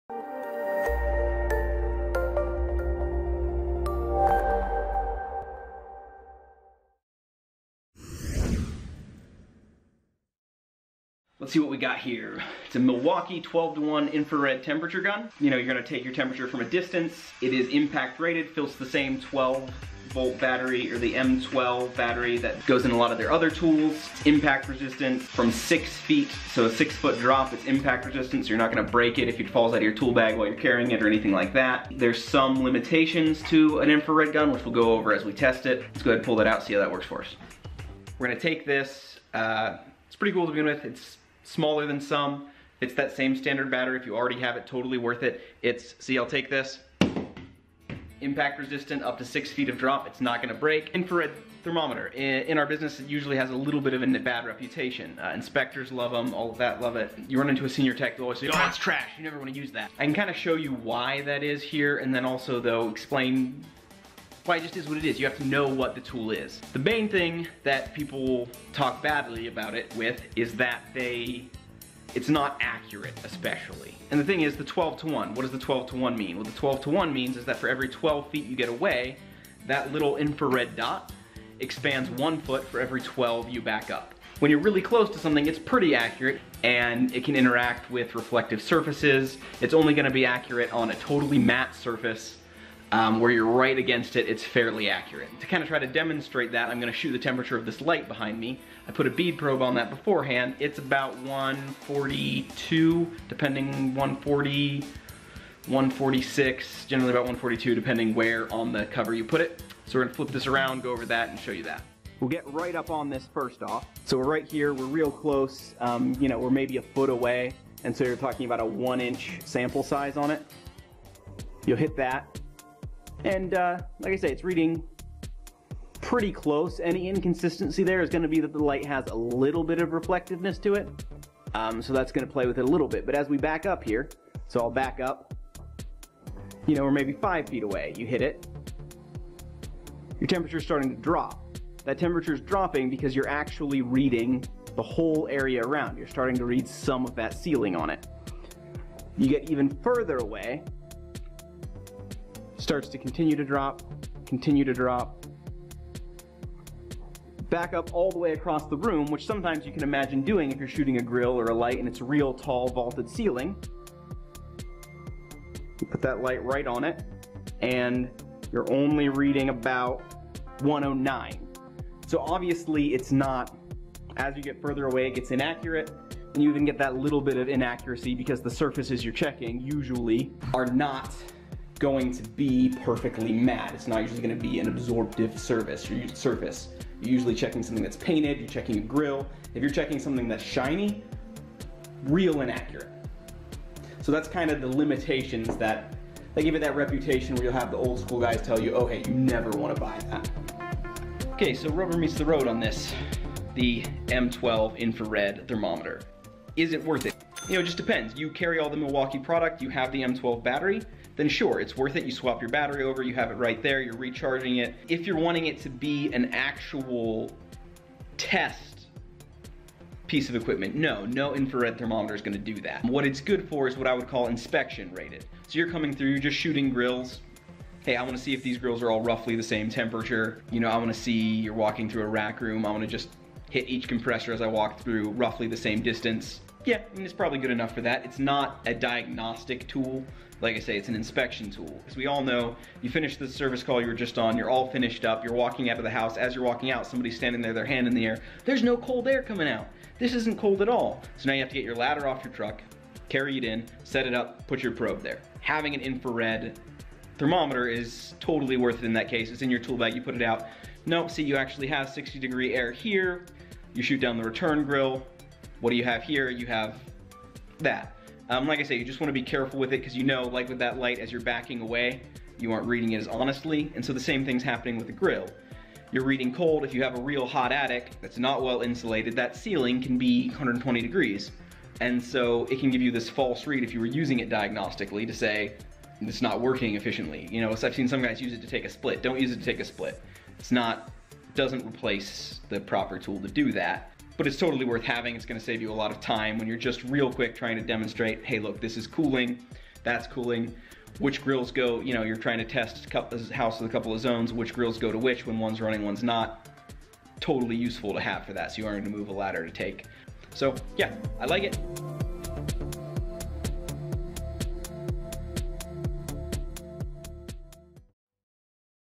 HVAC Tool Review - Milwaukee Laser TEMP-GUN Thermometer With Nathan Orr
In this video, Nathan Orr from Kalos Services & HVAC School gives his thoughts about the Milwaukee M12 TEMP-GUN infrared thermometer.

The Milwaukee TEMP-GUN is compact and is meant to measure temperature at a distance. Although inspectors tend to love it, it gets a bad rap from a lot of technicians. Some people slam the accuracy, but they have to understand that the temperature gun is affected by reflected surfaces and that the measurement range grows with distance. In the case of this video, as Nathan backed up from the light, the temperature reading dropped because the temp gun began picking up the ceiling temperature as the reading range increased with distance.

Overall, the M12 infrared thermometer is worth it if you plan to use it correctly and are aware of its limitations. If you are looking for a very accurate measurement, then you'll want to avoid infrared thermometers in general due to their limitations. It's an inspection tool, not a diagnostic tool.